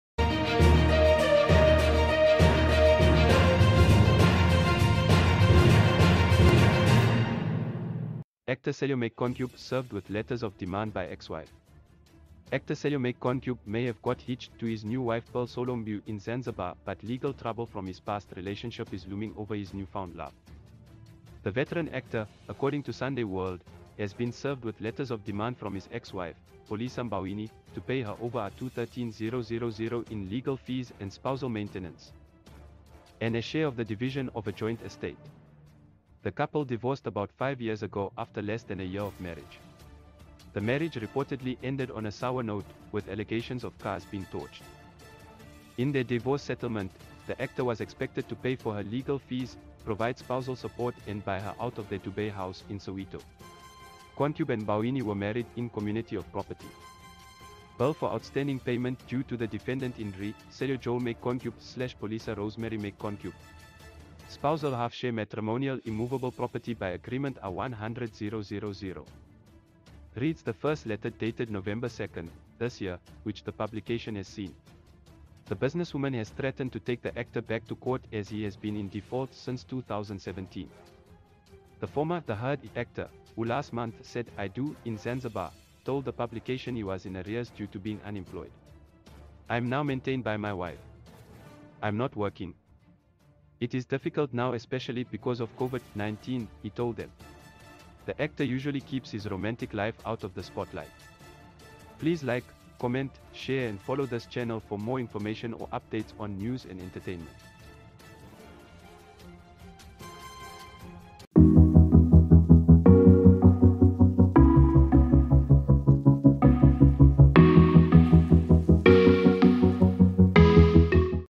Actor Sello Maake kaNcube served with letters of demand by ex-wife
Actor Sello Maake kaNcube served with letters of demand by ex-wife - Actor Sello Maake kaNcube may have got hitched to his new wife Pearl Solo Mbewe in Zanzibar but legal trouble from his past relationship is looming over his newfound love.

The veteran actor, according to Sunday World, has been served with letters of demand from his ex-wife, Palesa Mboweni, to pay her over R213 000 in legal fees and spousal maintenance, and a share of the division of a joint estate. The couple divorced about five years ago after less than a year of marriage. The marriage reportedly ended on a sour note, with allegations of cars being torched.

In their divorce settlement, the actor was expected to pay for her legal fees, provide spousal support and buy her out of their Dube house in Soweto. KaNcube and Mboweni were married in community of property.